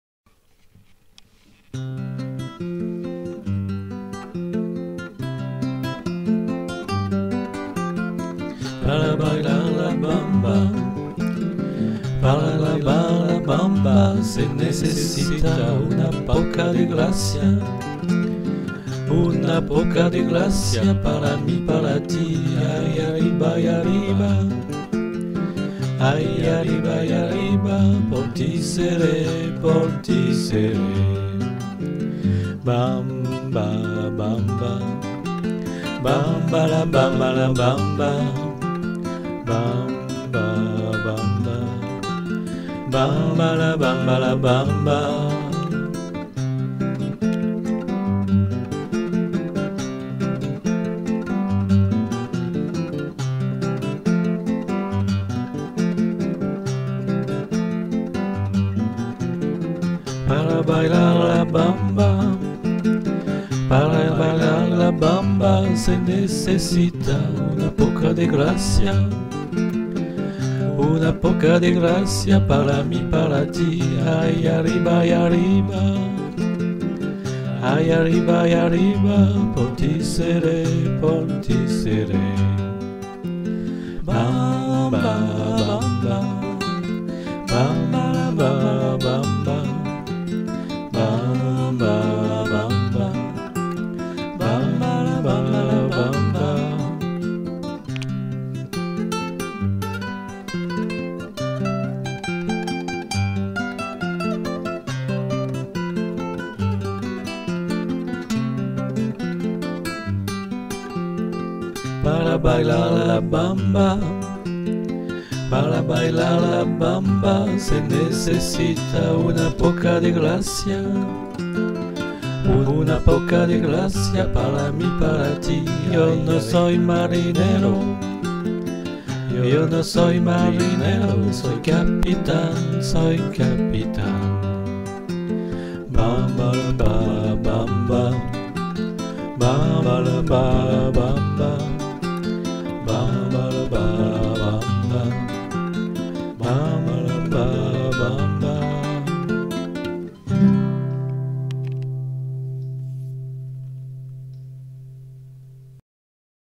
Murs peints
Montage photos où on voit beaucoup Nantes, Rezé, Saint Herblain et un peu l'Irlande et la Hollande.
Eric : guitares + 2 voix